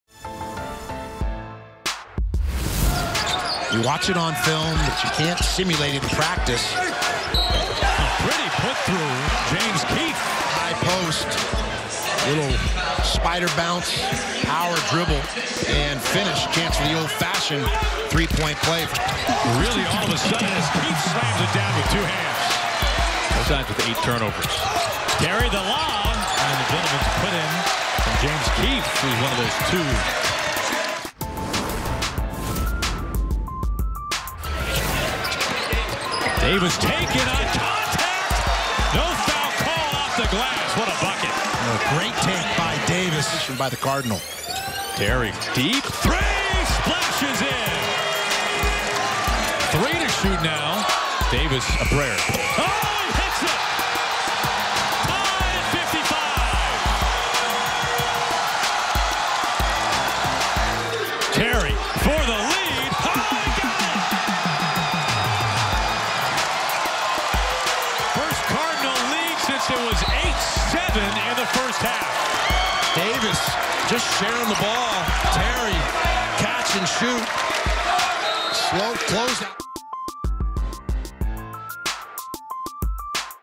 Stanford Men's Basketball vs. Washington [1.9.20]
Stanford remained unbeaten in conference play Thursday night with a massive comeback win over Washington. The Cardinal trailed by as many as 12 points, but ended the game on a 14-1 run to hand the Huskies their second conference loss of the season. Spencer Jones led the way for the Card, going 4-9 from three point range for a total of 14 points on the night. Daejon Davis and Tyrell Terry knocked down key three-pointers down the stretch to secure the victory, and to make Stanford the only remaining unbeaten team in Pac-12 play this season.